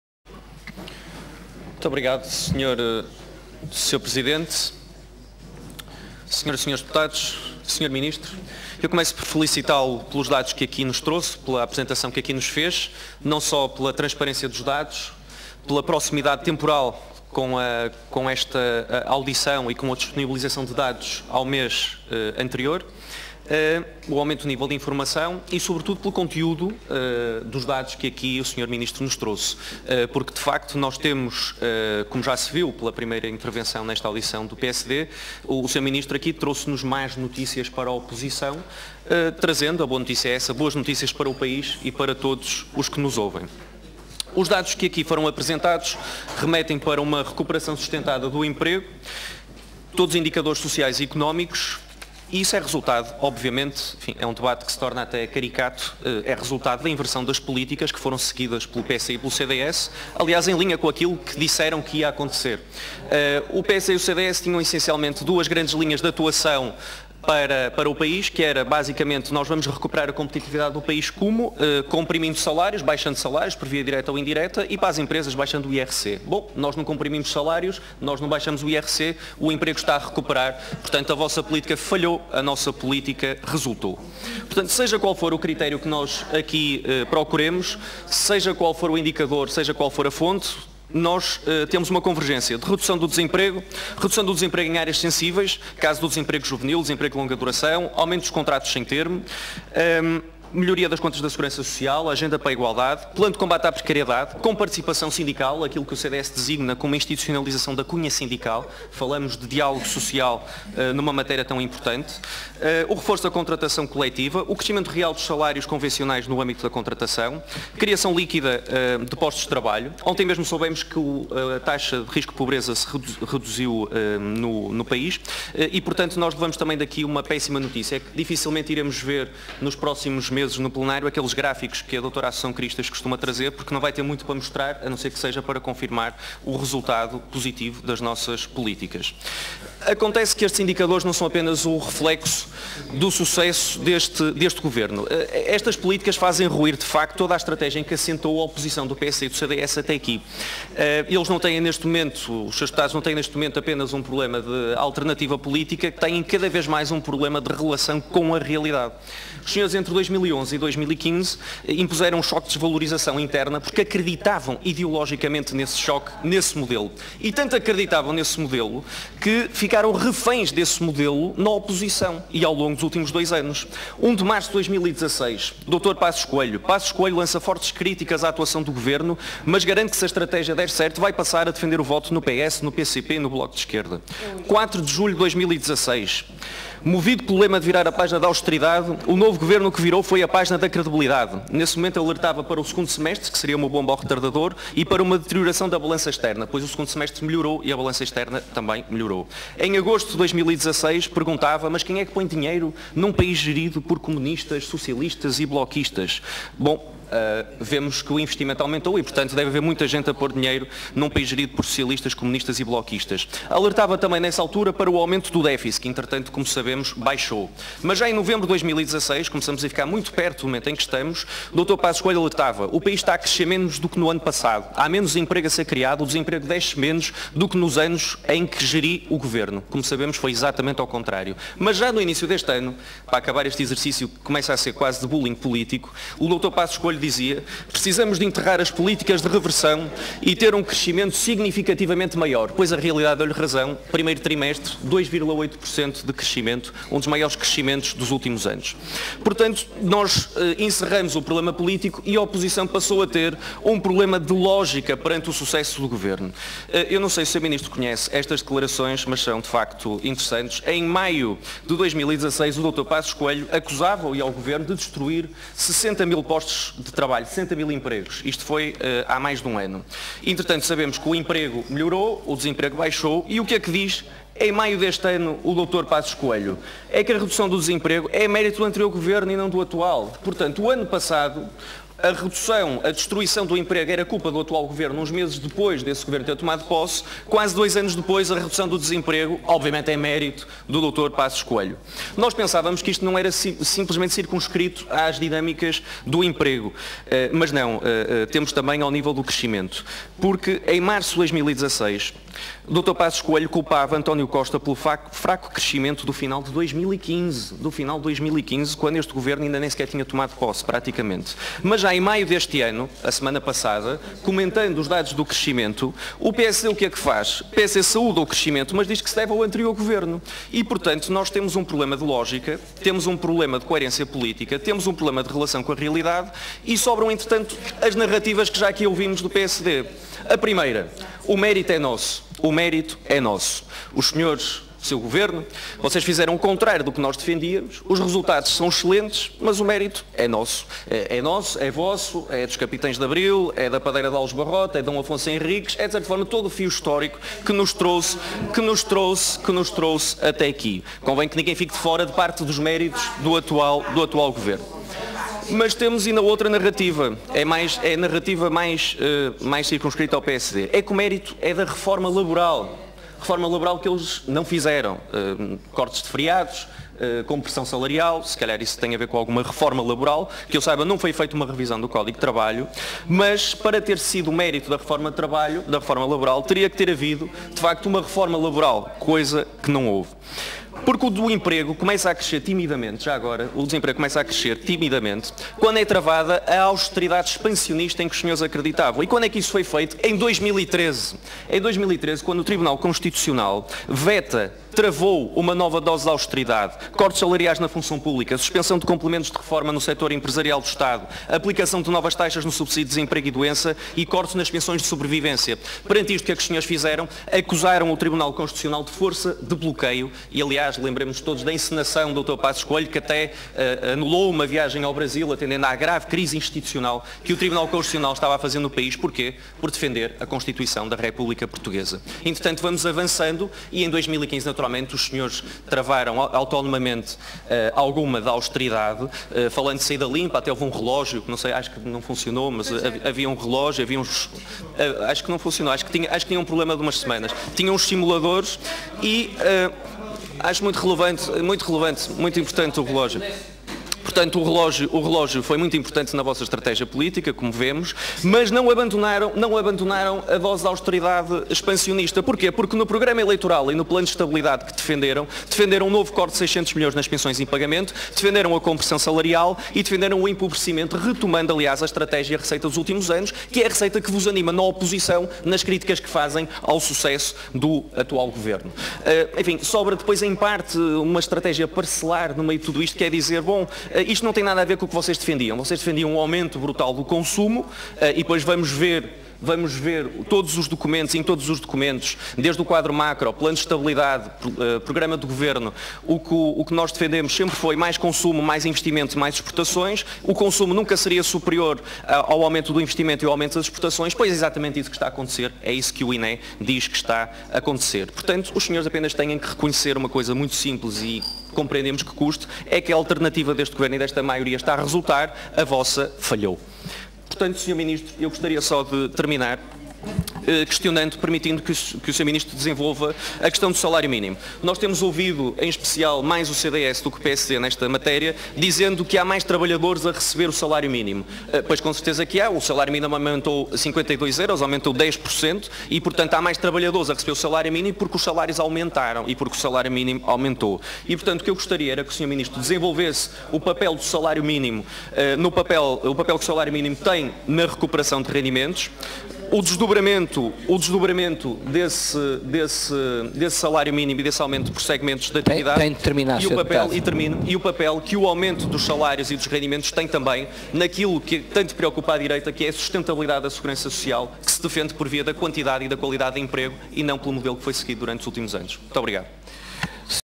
17-05-2017 | Audição do Ministro do Trabalho | Tiago Barbosa Ribeiro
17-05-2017 | Intervenção do Deputado Tiago Barbosa Ribeiro - Audição do Ministro do Trabalho, Solidariedade e Segurança Social Vieira da Silva - Comissão de Trabalho e Segurança Social